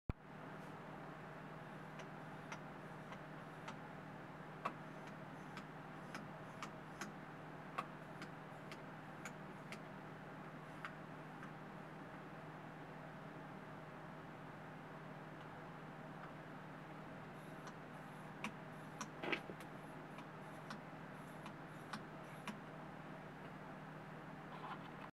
Repair DJ Gear | Traktor S2 Fader Issues (FIXED)
Repair DJ Gear - Traktor S2/S4 Specialists.
Located in Orange County, California.

This Traktor S2 had severe fluid damage. Ingress of fluid caused irreversible damage to all tempo and channel/cross faders. The damage also affected two LED micro switches. Lastly, the USB data circuits were shorting to ground due to corrosion.

We managed to diagnose the issues and offer repairs at affordable rates.

We HIGHLY recommend upgrading channel faders to the innofader.